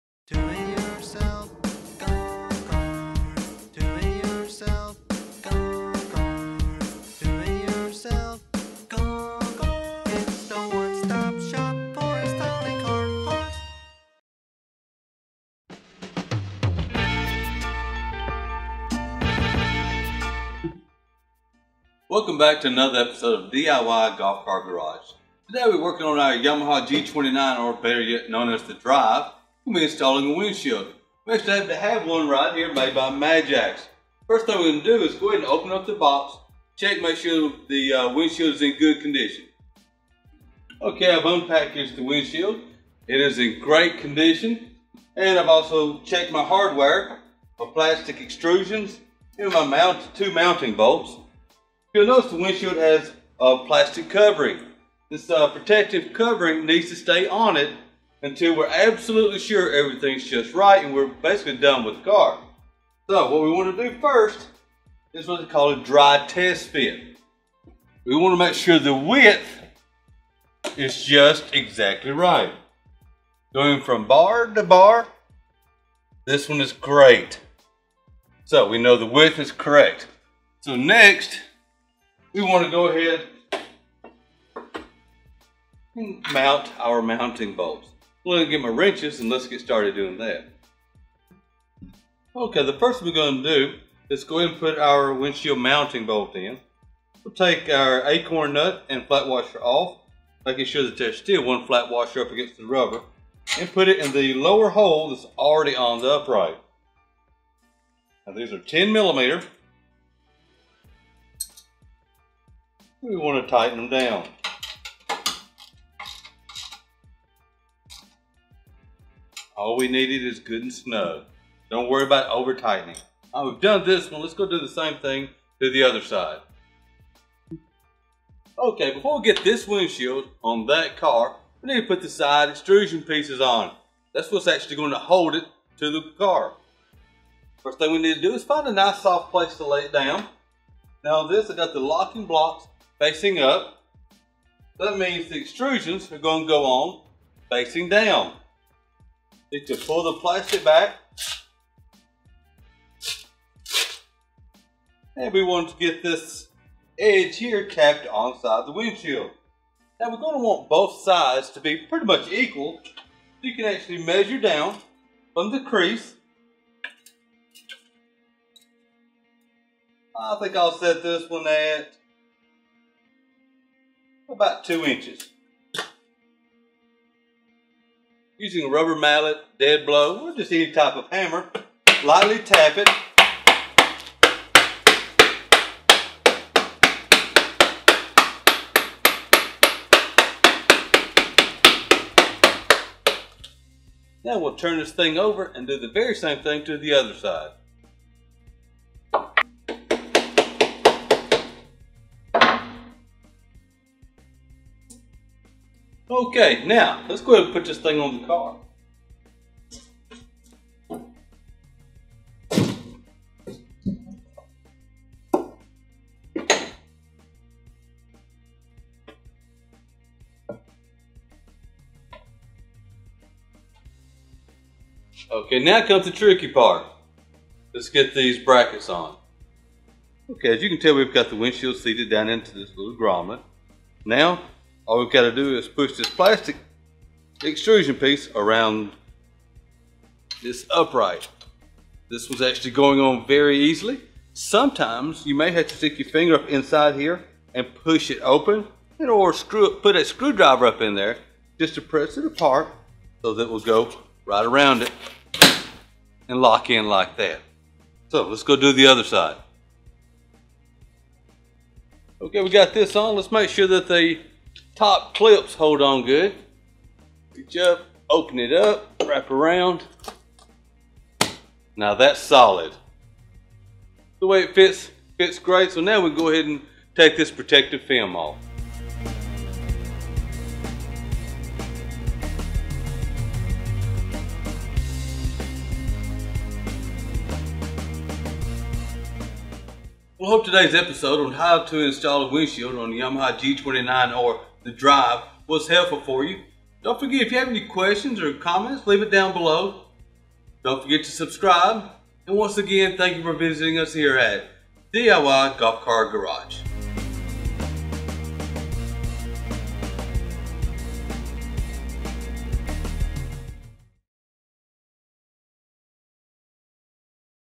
Yamaha G29 Drive Tinted Windshield | How to Install on Golf Cart
shows you how to install a tinted folding windshield on a Yamaha G29 "The Drive" golf cart. This tinted flip windshield allows you to enjoy the breeze in the summer months by folding it down while also allowing you to block the cold frigid air in the winter months by flipping it up. We carry windshields for all makes and models.